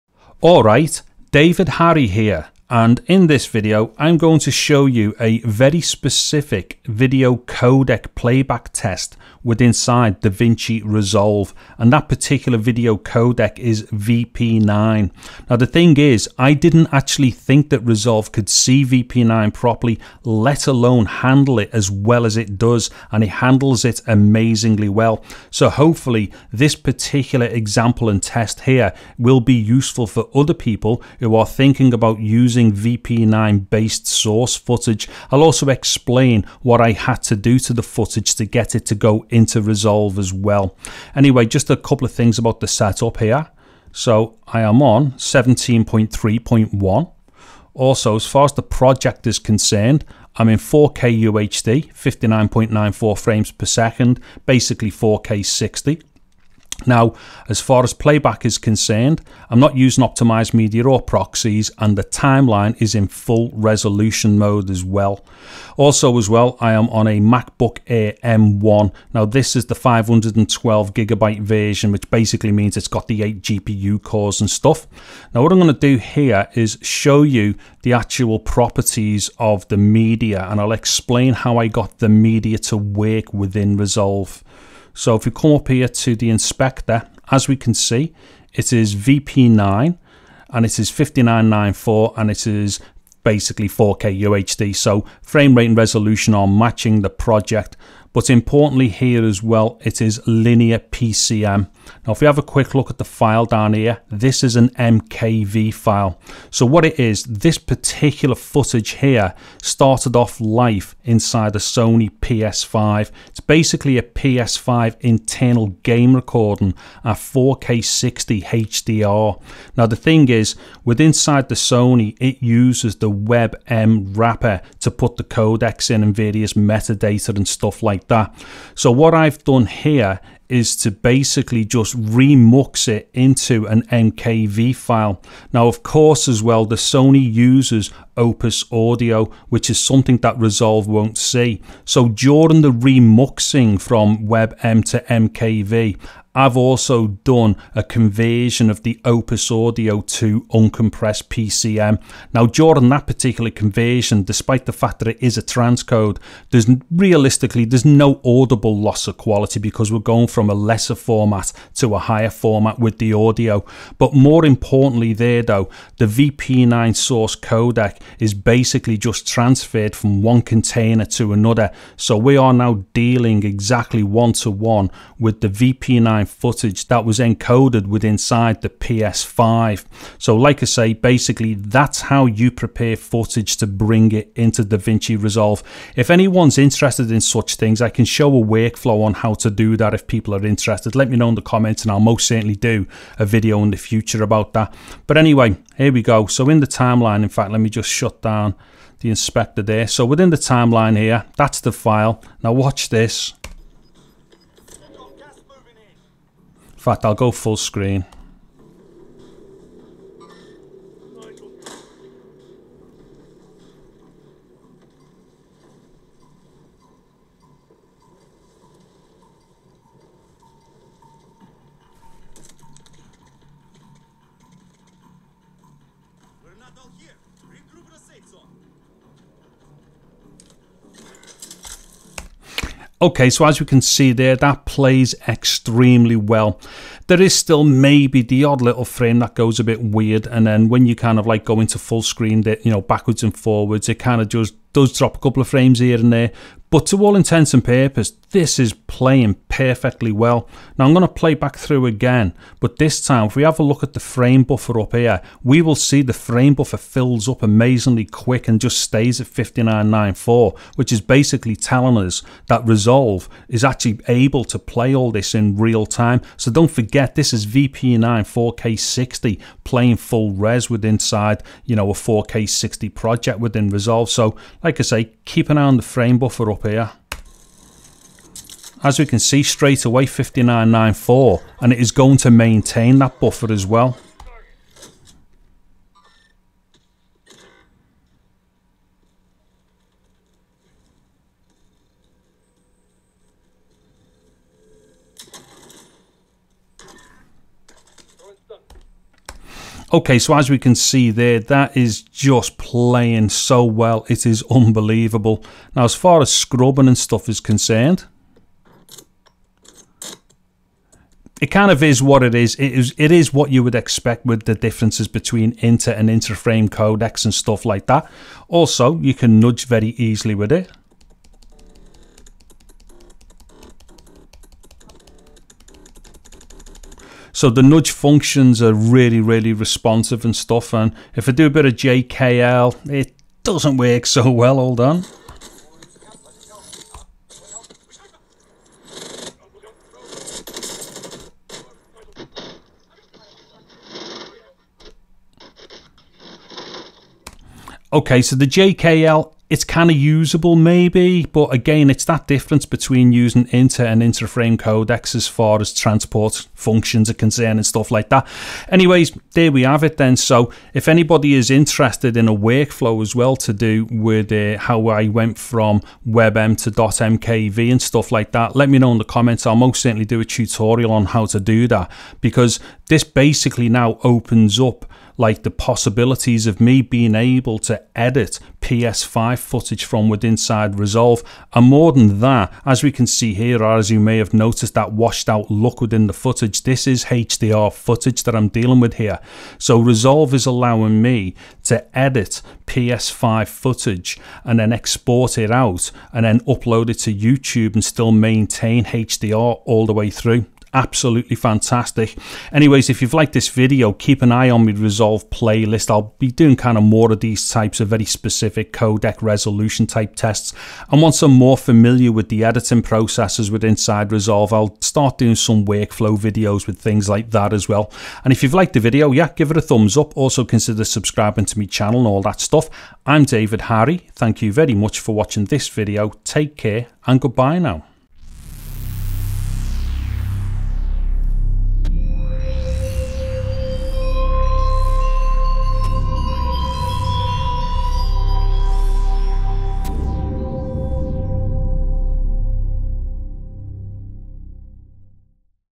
DaVinci Resolve 17.3.1 Playing VP9 & MKV On MacBook Air M1 - Sony PS5 4K 60FPS HDR WEBM To MKV Remux
This video is showing how well DaVinci Resolve is playing back the VP9 video codec from within an MKV file. MKV is the Matroska codec wrapper/container and also the .mkv file extension.

The computer used was the Apple MacBook Air M1 with 8GB RAM and 512GB storage. Being the 512GB storage version, this also means that it has the 8 GPU cores on its Apple Silicon M1 processor/SoC. As the MacBook Air has the worse thermal throttling characteristics of all the Mac M1 computers to date. This means that what I'm doing in this video will be at least as good, possibly better, on the Mac Mini M1, MacBook Pro M1 and the iMac M1.

The video footage was sourced from the Sony PS5. This is gameplay capture video that the PS5 records internally. The gameplay and recording on the PS5 was 4K 60FPS and in HDR. This forces the PS5 to use the WEBM container/wrapper to hold the VP9 HDR 4K 60FPS video codec/stream and also a stereo audio track that is in the Opus audio codec format.

Because Resolve can't import WEBM files and also can't import and decode the Opus audio codec format, a re-mux is necessary. The re-mux was done using FFmpeg with a GUI frontend. During the re-muxing the VP9 video stream is transferred straight to the MKV file from the WEBM file. The Opus audio stream is transcoded to PCM so that Resolve is able to see/decode the audio. While the audio is a transcode, it is audibly indistinguishable compared to the source. The most important thing here is that the we are dealing with the native VP9 4K 60FPS HDR video stream that the PS5 encoded. Out of interest, the WEBM file was about 4.5GB in size and took about 15 seconds to re-mux to MKV, this includes the audio transcode.

Although I didn't do an edit and export in this video, I have edited this footage in Resolve, exported a 4K 60FPS HDR H.265 MP4 and used it to upload to YouTube. The final upload on YouTube plays back in HDR and also has SDR conversions as well.

Interestingly, I also have an Atomos Ninja V and have captured the PS5's HDMI output to it to record 4K 60FPS HDR to ProRes 422 HQ. This footage has then been edited in Resolve and uploaded as a HDR master to YouTube in H.265. Having looked at both workflows, it does appear that the MKV version of the workflow doesn't look visually any different to the Ninja workflow. This may sound counterintuitive, it did to me, but my tests show that there really isn't any need to record the PS5 to a HDMI recorder like the Ninja. This may well be due to the fact that both workflows use heavily compressed uploads for YouTube. And although the Ninja workflow includes 4:2:2 chroma-subsampling, again, it didn't seem to have any advantages over the MKV workflow. However, if you were to upload a ProRes 422 HQ master from Resolve from the Ninja workflow, I'm sure this would give technical benefits. However, such a file would run into the hundreds of megabytes, depending on content runtime, and would be highly impractical for most people.

You can of course do editing from within the PS5, although its editing software, as good as it is for what it is, is nowhere as flexible as editing in Resolve or any other pro NLE.

I will be doing a full workflow video about the whole process to go from the PS5, to Resolve and then to YouTube soon.

While I am new to Resolve and still learning to use it as my main NLE, migrating from Edius X by Grass Valley. My codec, resolution and frame rate tests may be useful to others. Once I'm happy that I'm at a sufficient level with Resolve's NLE functions, I will start doing editing workflow videos.

Cheers,
Dave.